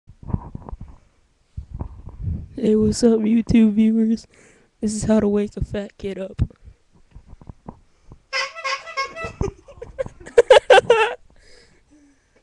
How to wake a fat kid up
we wake up ruthless when he is sleepin